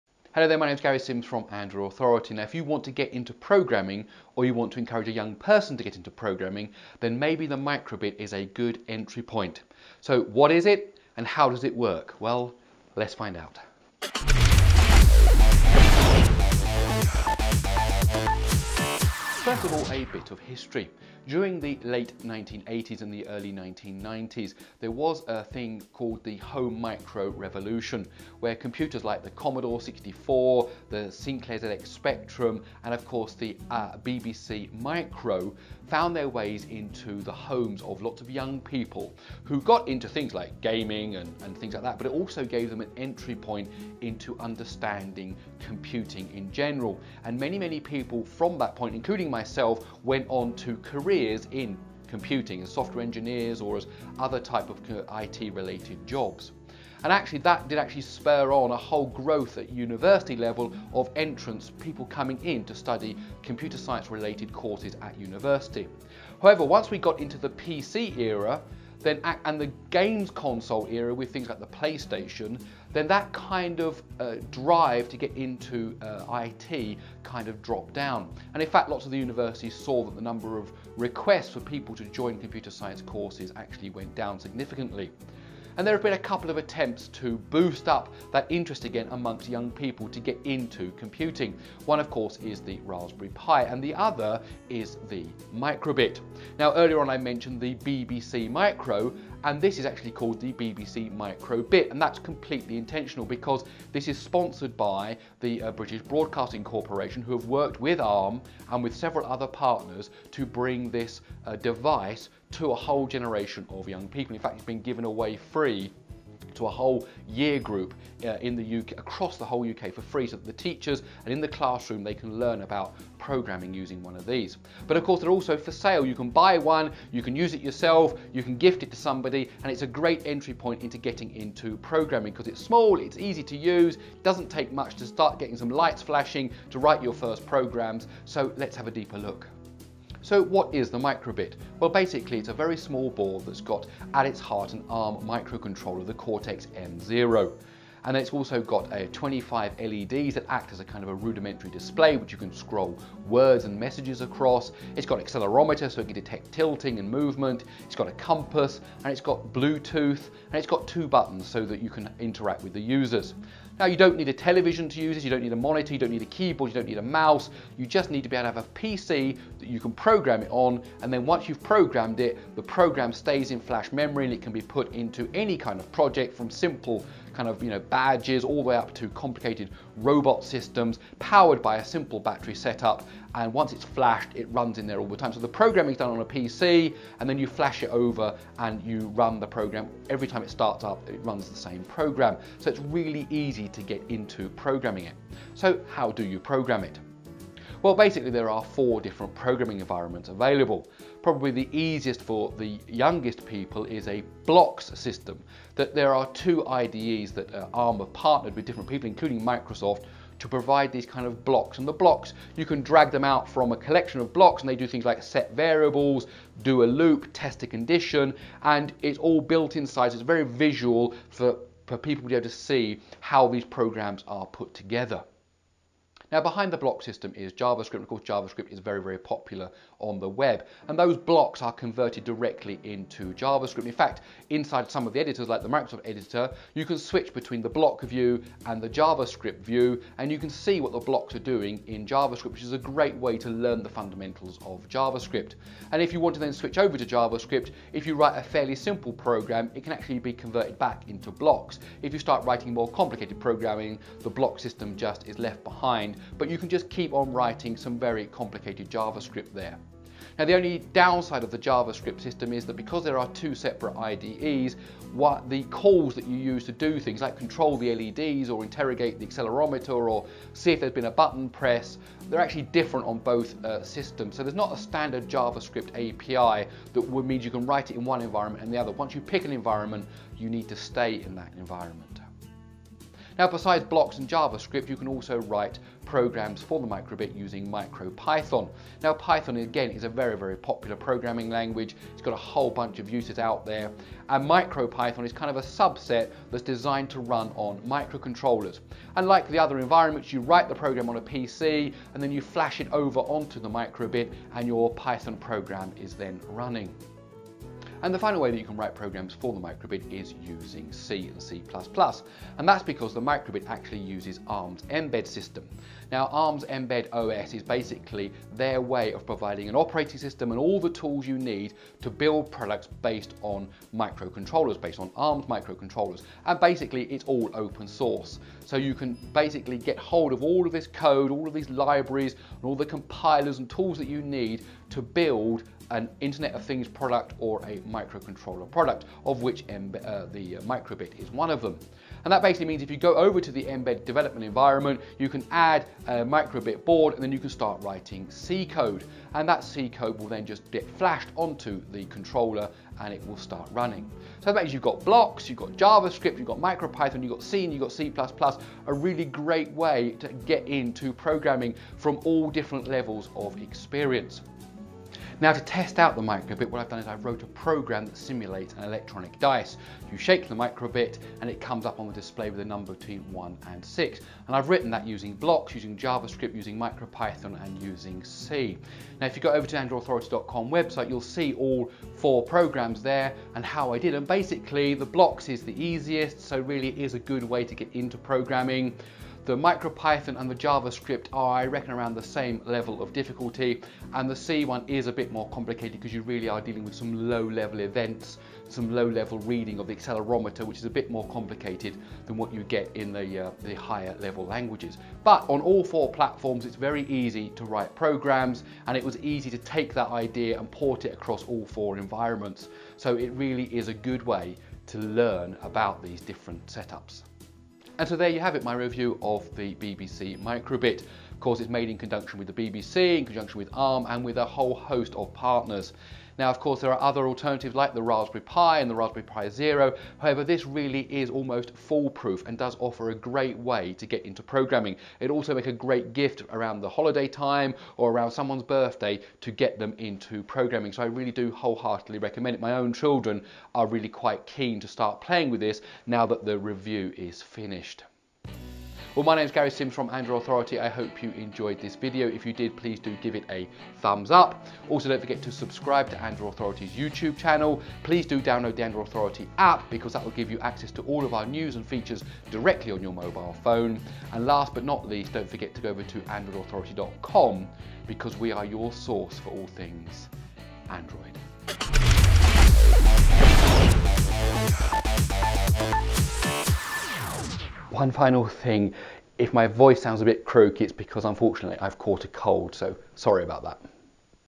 What is the BBC micro:bit - Gary explains
The micro:bit is a small computer board with an ARM Cortex-M0 microprocessor plus sensors and LEDS. It is part of an initiative to get kids coding. Interested? Here is my full review.